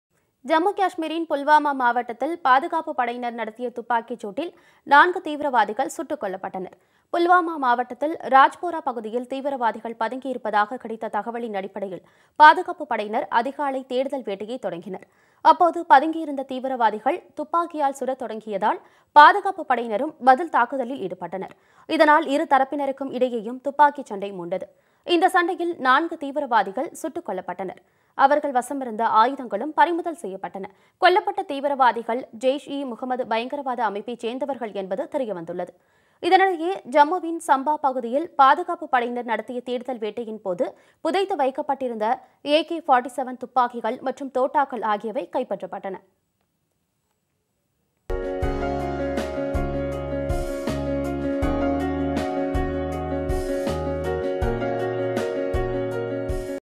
ஜம்மு-காஷ்மீரில் பாதுகாப்புப்படையினர் நடத்திய துப்பாக்கிச்சூட்டில் 4 தீவிரவாதிகள் சுட்டுக் கொலை
Four militants were shot dead by security forces in the Pulwama district of Jammu and Kashmir.
Based on the information that the militants were hiding in the Rajphora area in Pulwama district, security forces started searching early in the morning. The security forces were also involved in the attack because the terrorists who were hiding in the area began shooting.

This led to gunfire between the two sides. 4 fighters were shot dead in this battle. Their weapons were confiscated.

 The terrorists killed were identified as Jaish-e-Mohammad. Meanwhile, in Samba area of ​​Jammu
 AK-47 guns and bullets were seized during the search operation by security forces.